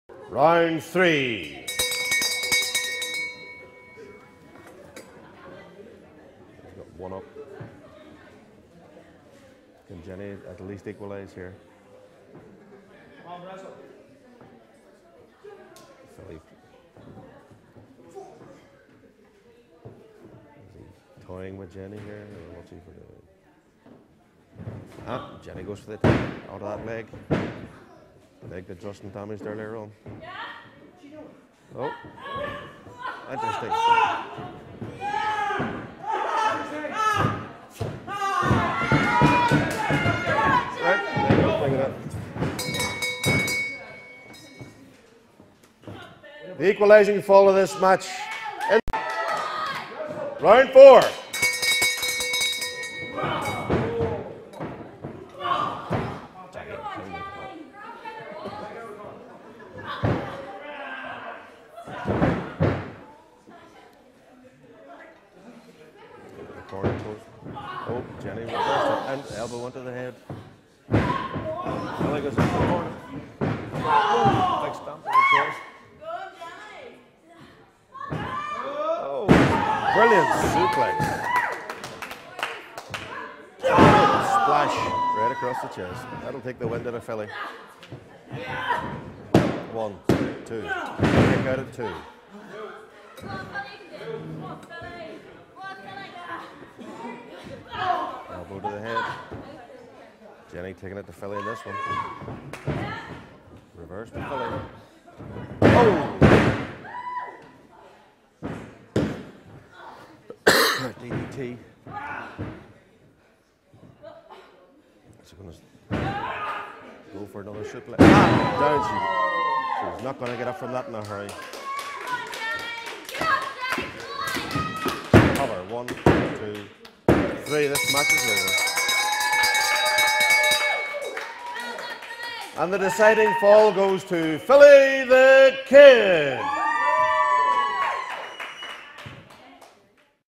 Jenny vs Philly rounds 3 and 4
Jenny quickly squares the match. All to play for in Round 4 !